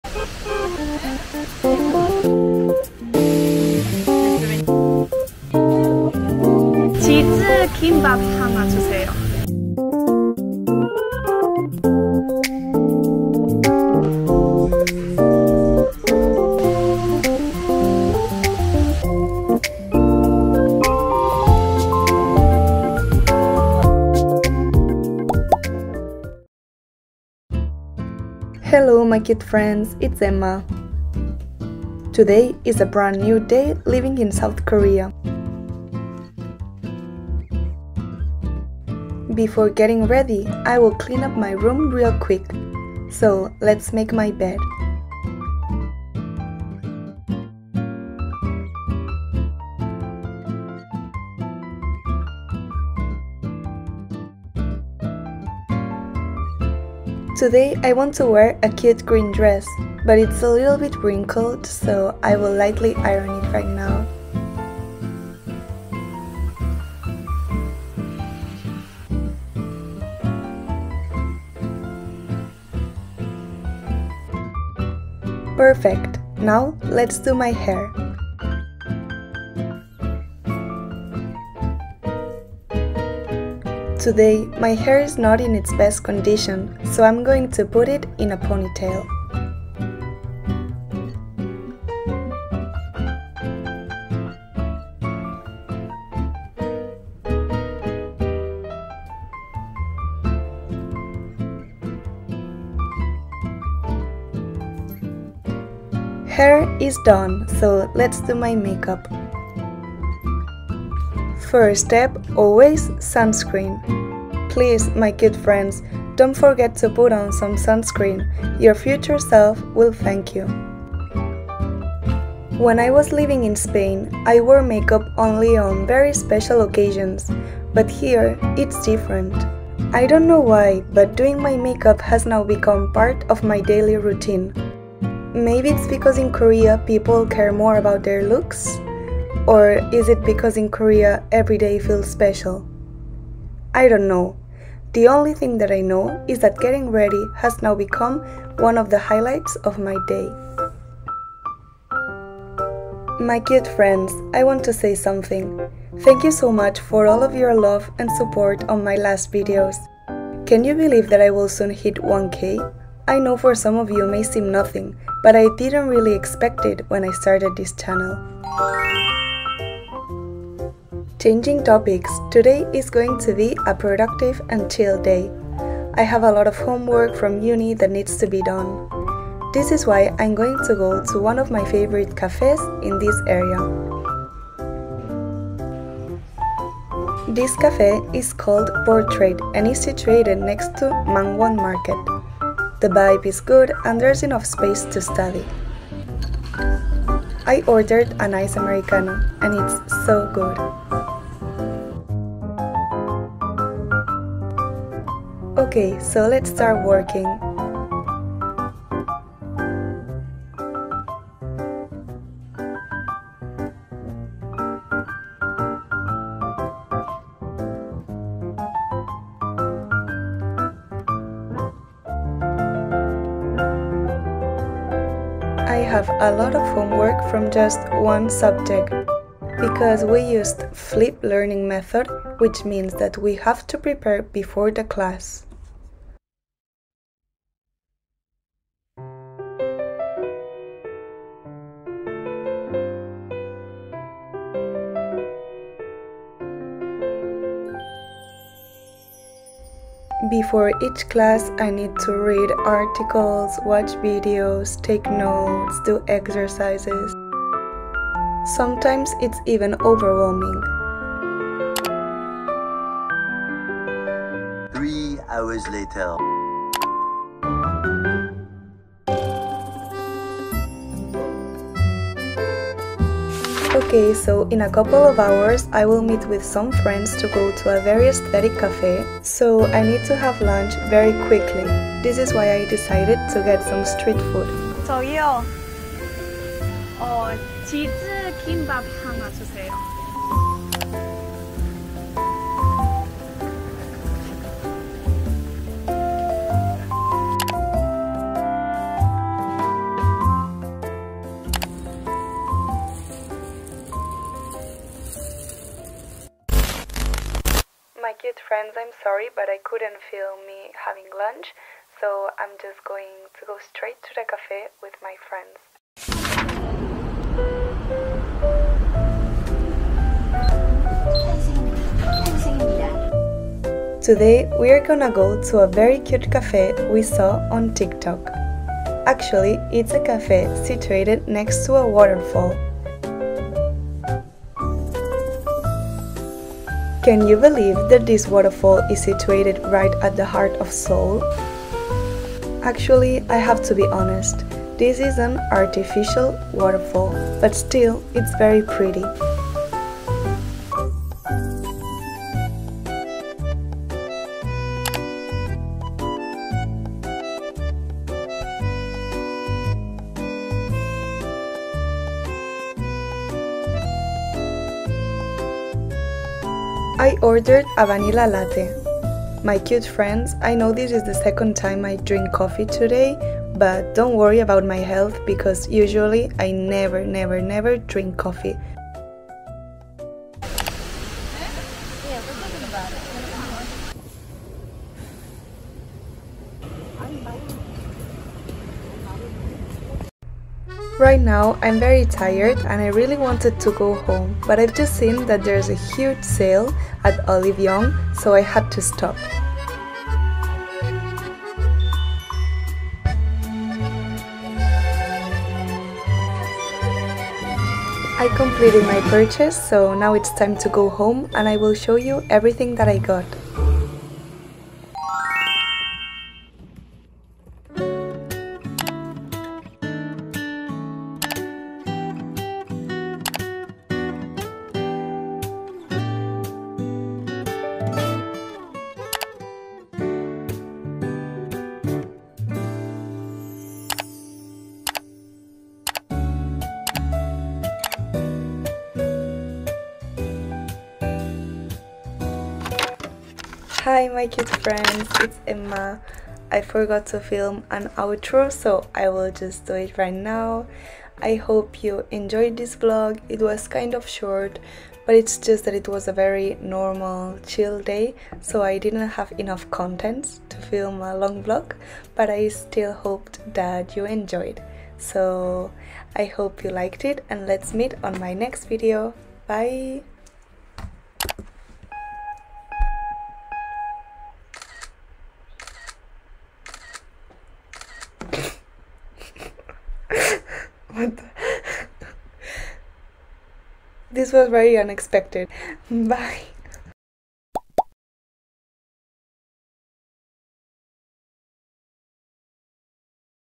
PRODUCTIVE KOREA VLOG 🇰🇷🍀 Cafe Hopping, Eating Street Food, Shopping at Olive Young and more✨
Hello my cute friends, it’s Emma ♡✧˖°

Long time no see! I’ve missed you ˚ʚ♡ɞ˚

Today it was a very chill day in South Korea. Soon I will have my midterm exams, and thus, I need to study and do a lot of homework!! Doing homework can be boring but… not when you do them in a cute cafe!! So that’s exactly what I did. I went to one of my favorite cafes in Seoul, located in Mangwon-dong, and spent more than two hours doing homework.

After finishing all of my homework I had a quick lunch and went to another café with my friends. Yeah… two cafes in only one day… I know it can seem like too much, but Seoul has so many cute cafes to explore!! The second café is located next to a waterfall. It is so beautiful!! Actually, it is very refreshing to see nature when living in a huge city like Seoul. I really loved being in that café and I spent more than two hours there.

After staying at this cafe, it was already time to go home. But… first…. I had to stop at Olive Young!! There was a huge sale going on and I couldn’t miss it!!

WAIT! I HAVE SOME DISCOUNTS FOR YOU 🥹 Make sure to check them out 🌷✨

/ づ♡ I love you

Time stamps

00:00 - Intro
00:26 - Morning Routine
02:49 - Study in Café
04:33 - Lunch Time
05:16 - Waterfall Café
06:30 - Olive Young

And finally, i'm going to leave some tags and keywords to boost the SEO (∗˃̶ ᵕ ˂̶∗)